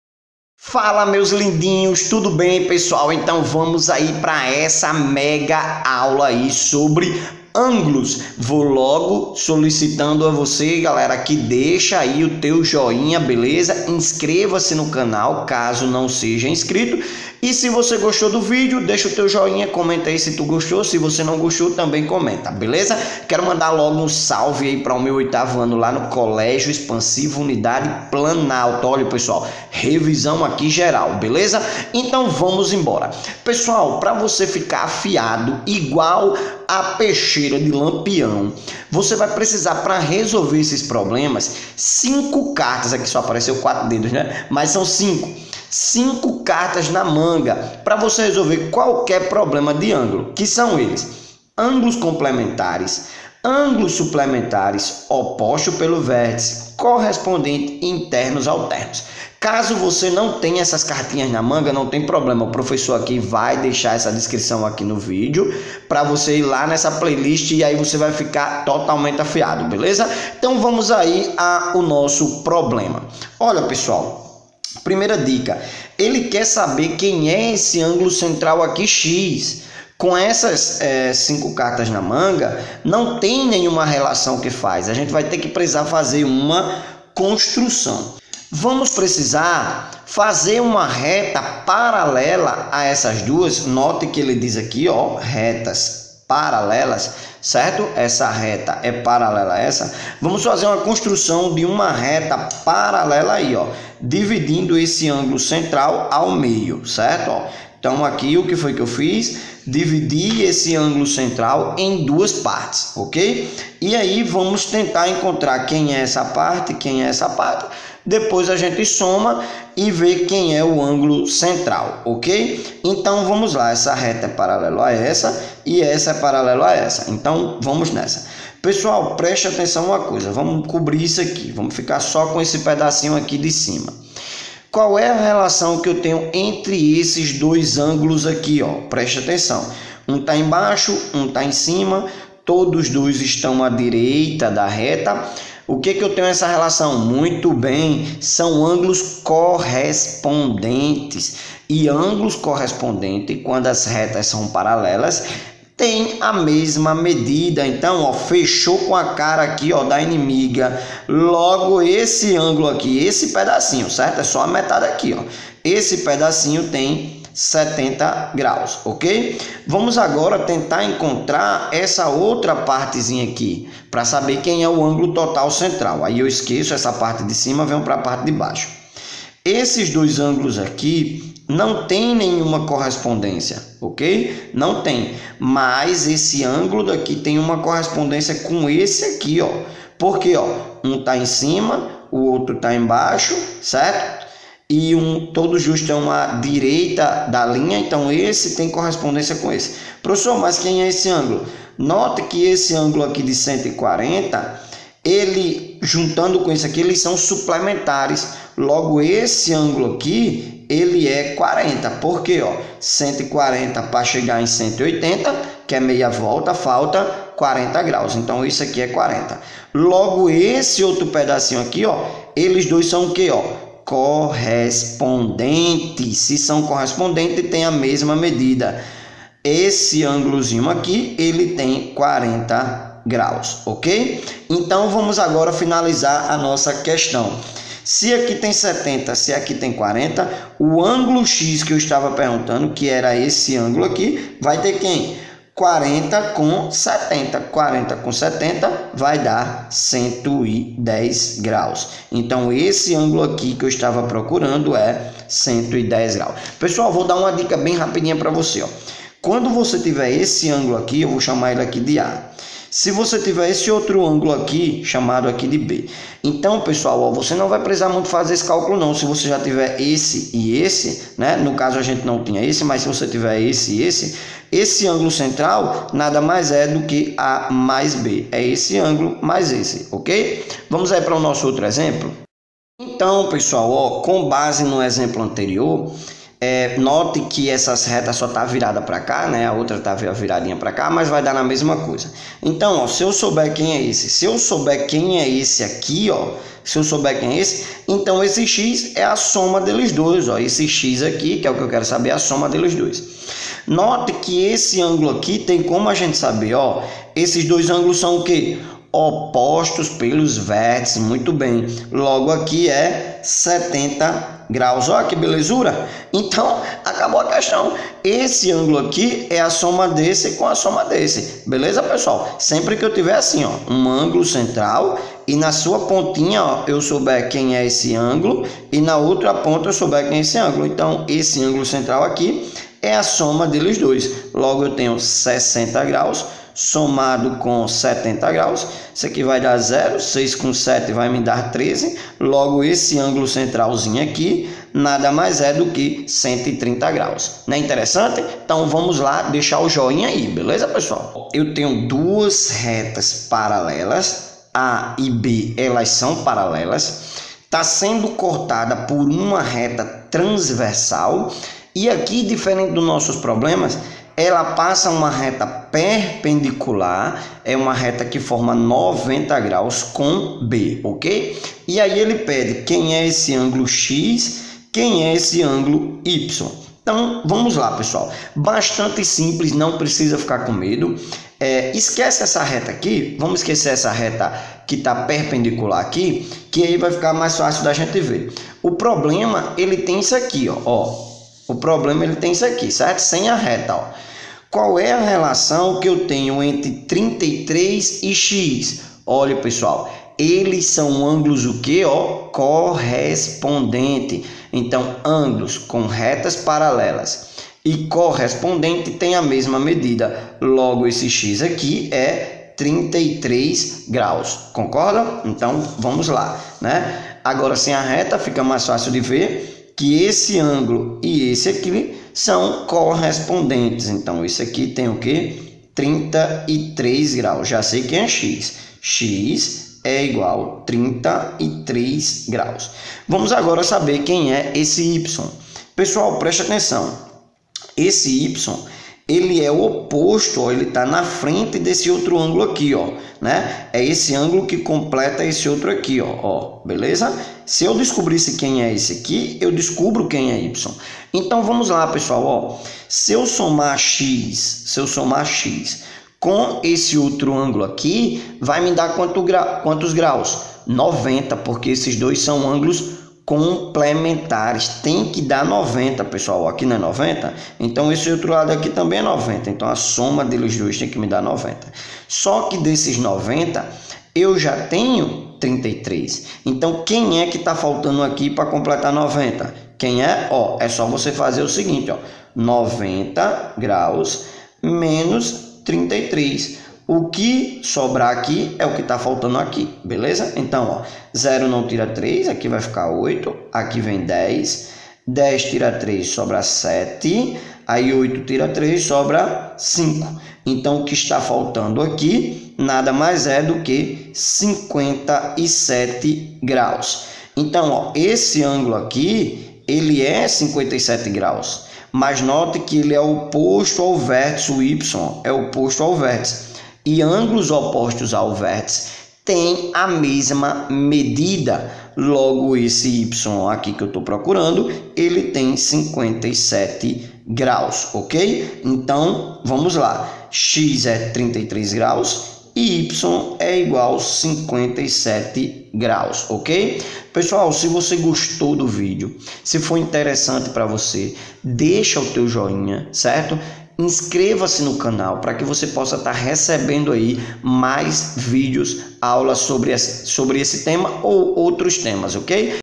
ÂNGULOS: EXERCÍCIOS RESOLVIDOS : Passo a Passo. Prof. Micamática.
Aprenda a resolver 3 questões de ângulos, onde temos duas retas paralelas e uma transversal.  contando também como uma super dica , de quando uma questão solicita, o ângulo central.

TUDO SOBRE ÂNGULOS.